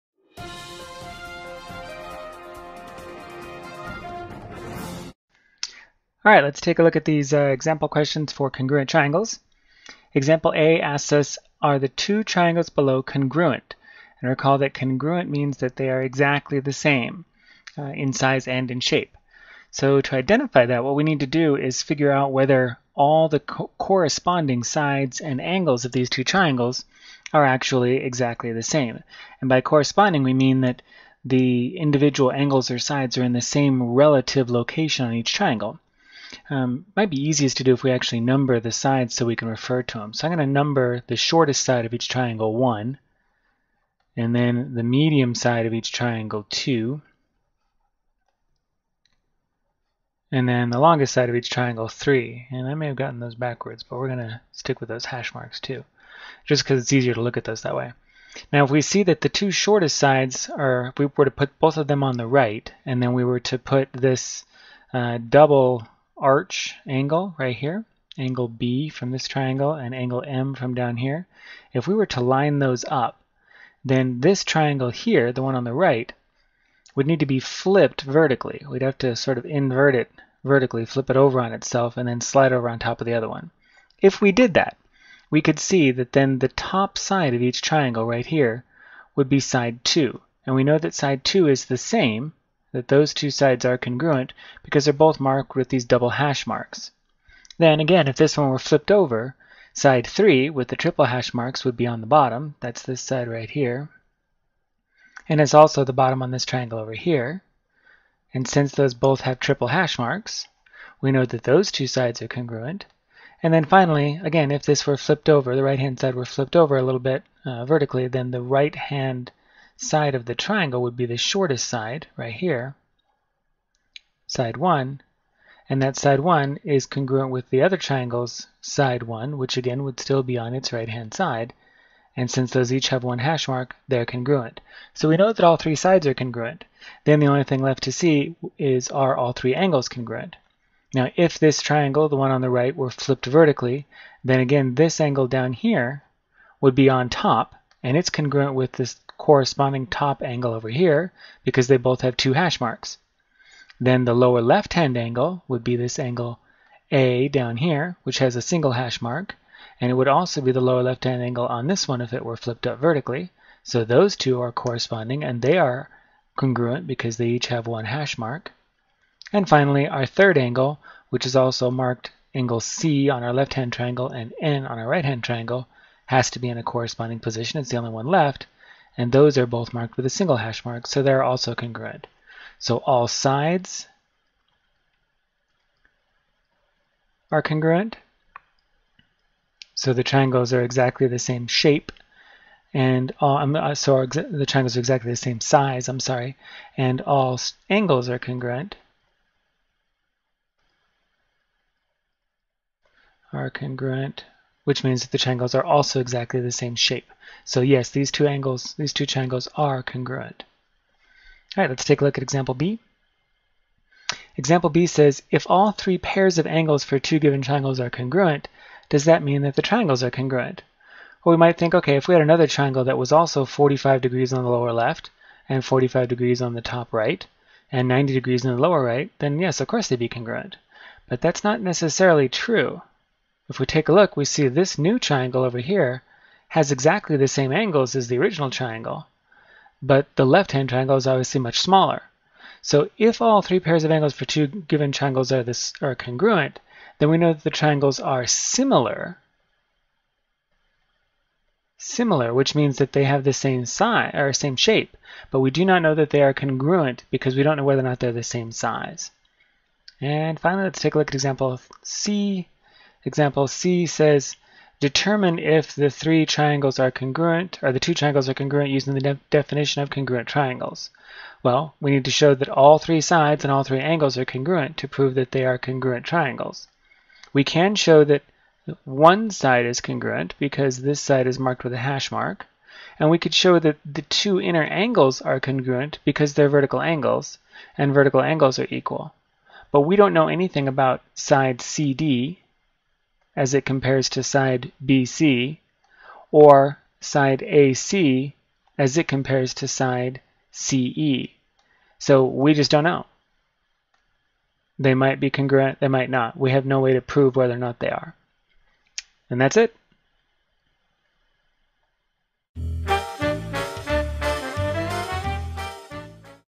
Congruent Triangles: Examples (Basic Geometry Concepts)
Here you'll learn that two triangles are congruent if they have exactly the same size and shape. You'll then use this fact to determine if two triangles are congruent.

This video provides the student with a walkthrough of one or more examples from the concept "Congruent Triangles".

This is part of CK-12’s Basic Geometry: Triangles and Congruence.